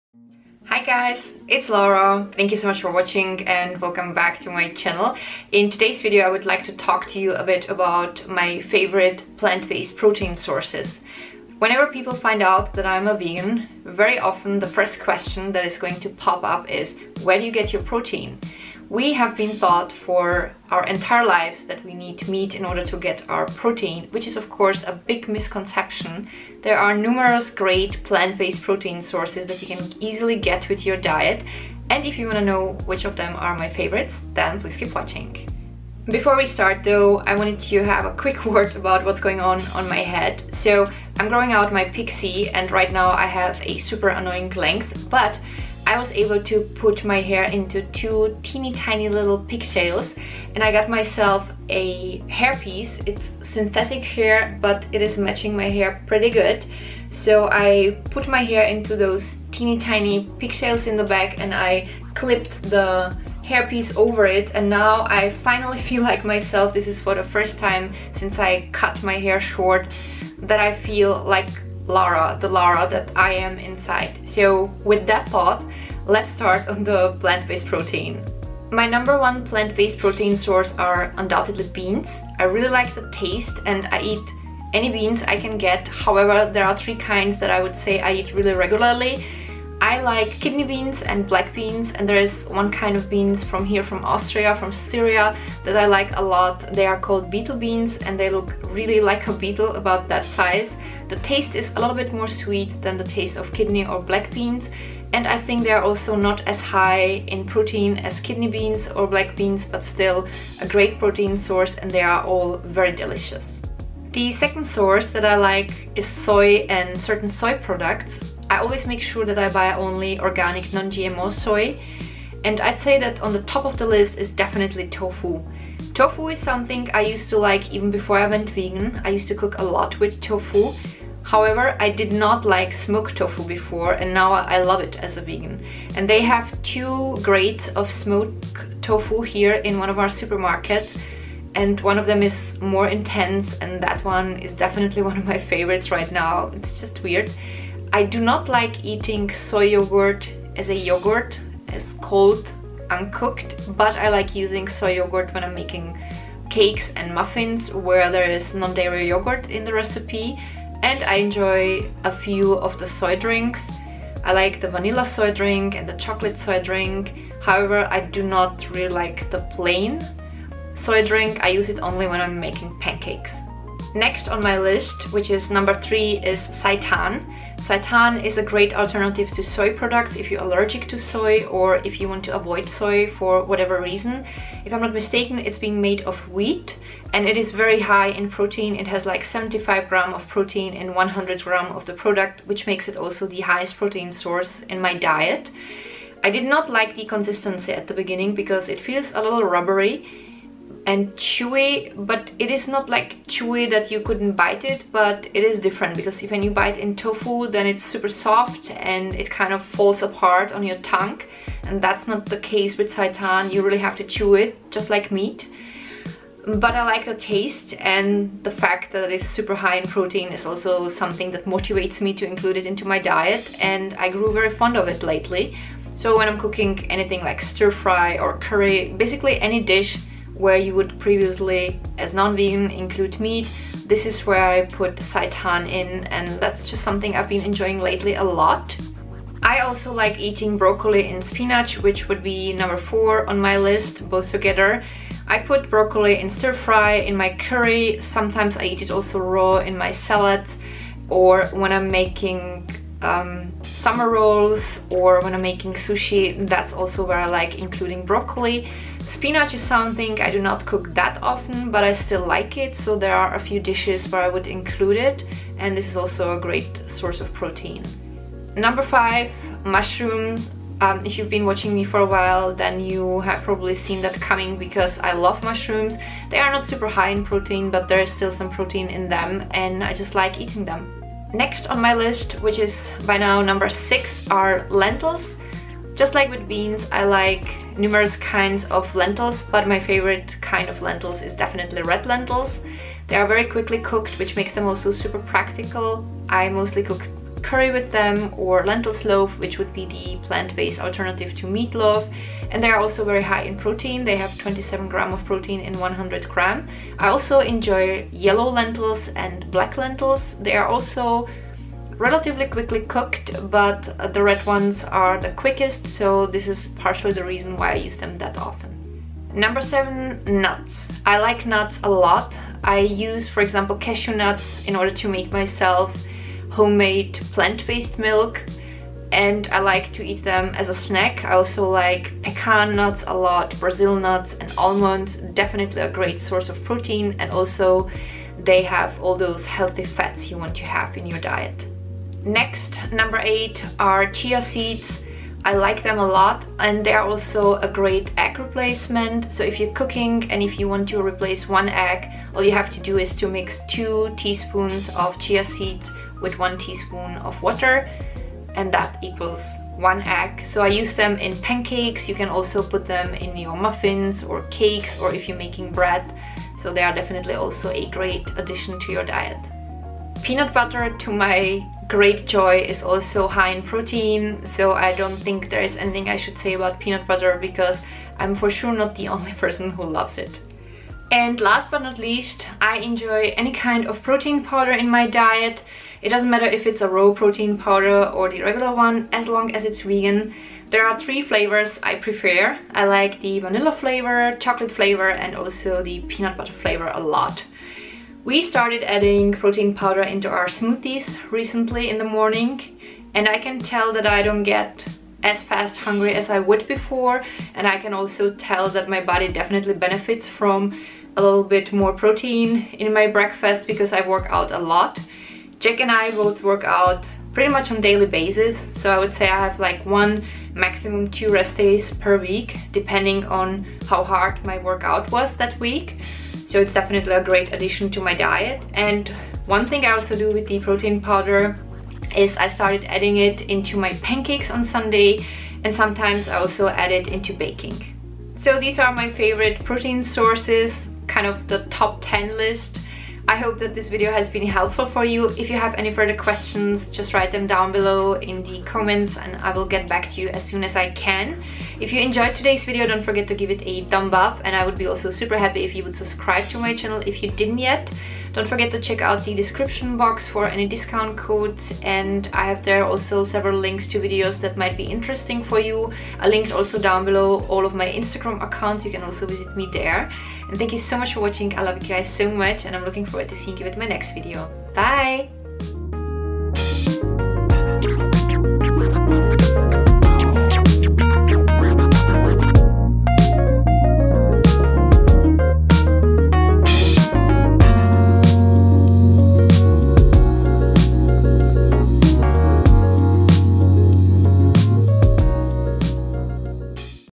Favorite Protein Sources | Fit Vegan Over 40
Today I will tell you what kind of plant-based protein sources are my favorite. Have fun!